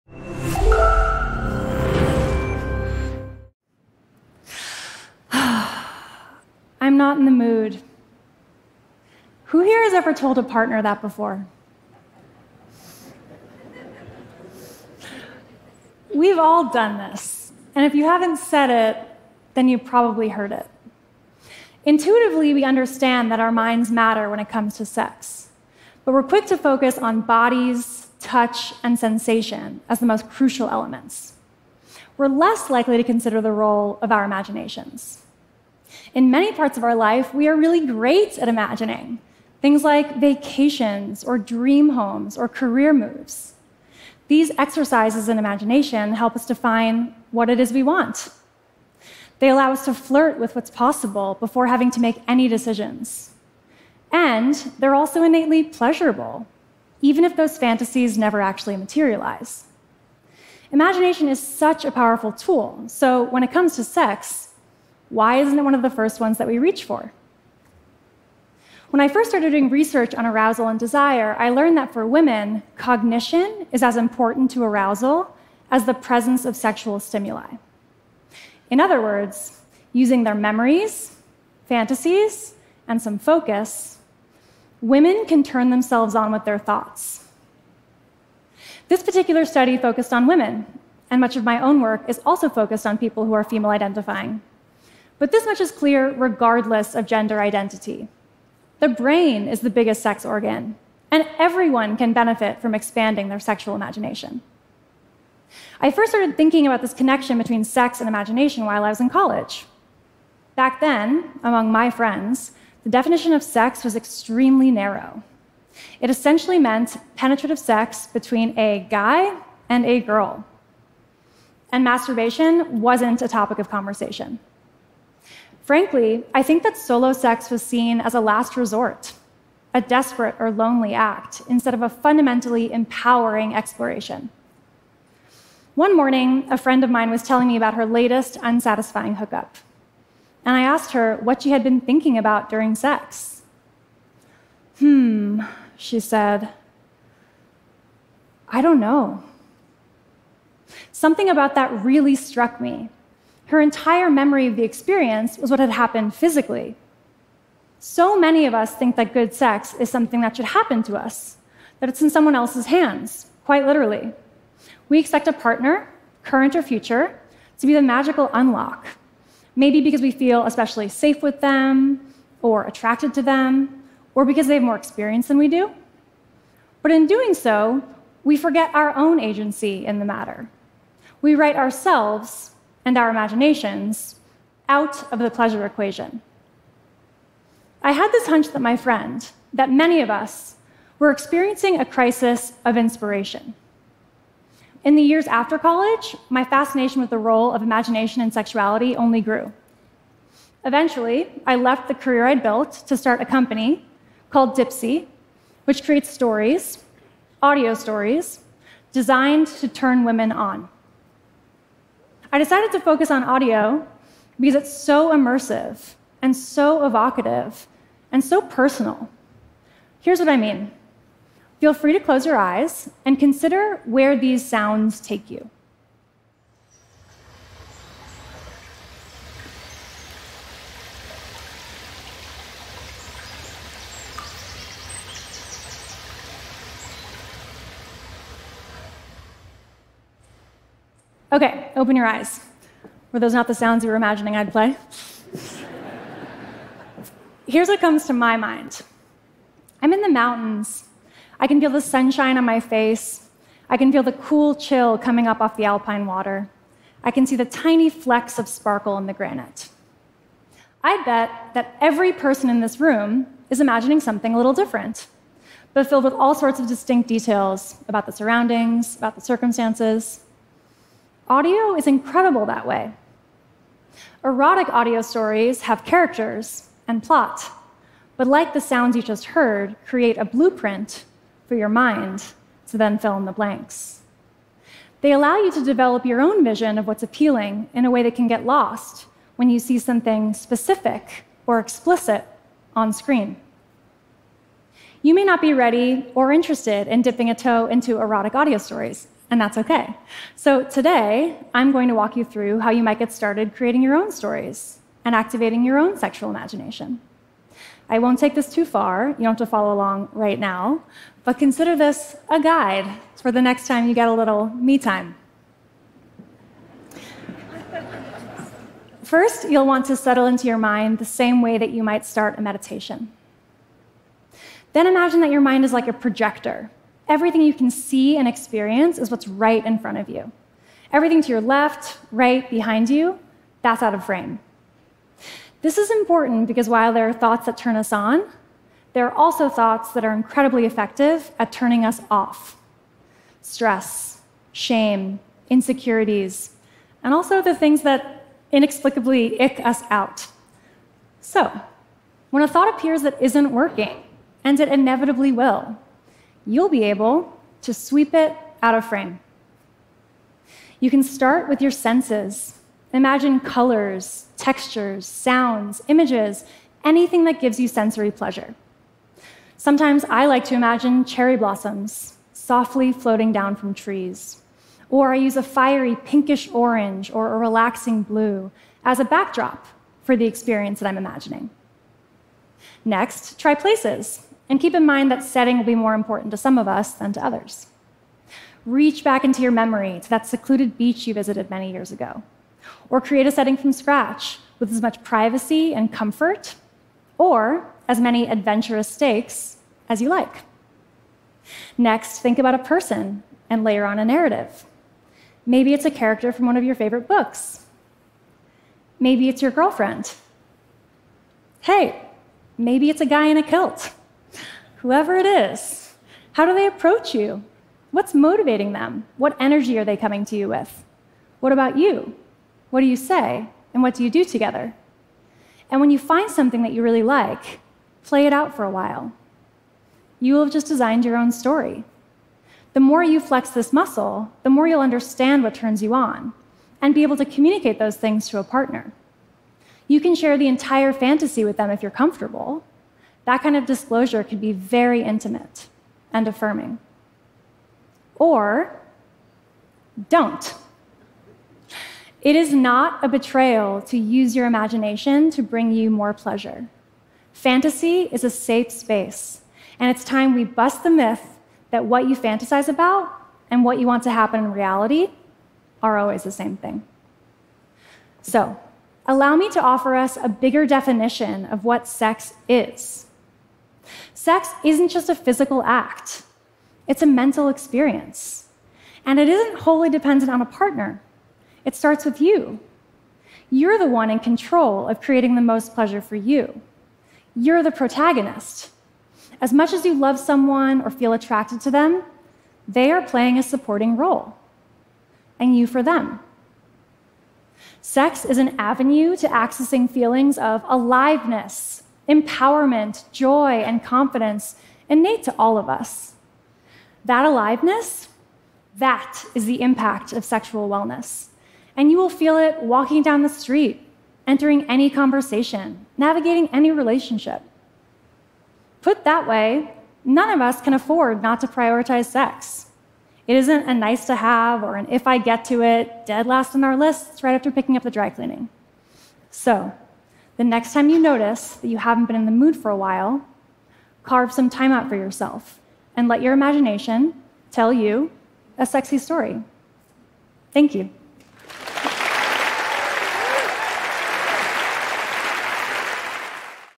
The Relationship Between Sex and Imagination | Gina Gutierrez | TED
Sex is as much mental as it is physical -- and imagination is the most powerful tool we have to expand our personal agency and capacity for pleasure, says sexual wellness storyteller Gina Gutierrez. The founder of audio-erotica company Dipsea, Gutierrez creates immersive audio stories designed to open up space to explore your desires and fantasies on your terms. She shares some tips to inspire your sexual imagination and bring joy, confidence and empowerment into your life.

Follow TED!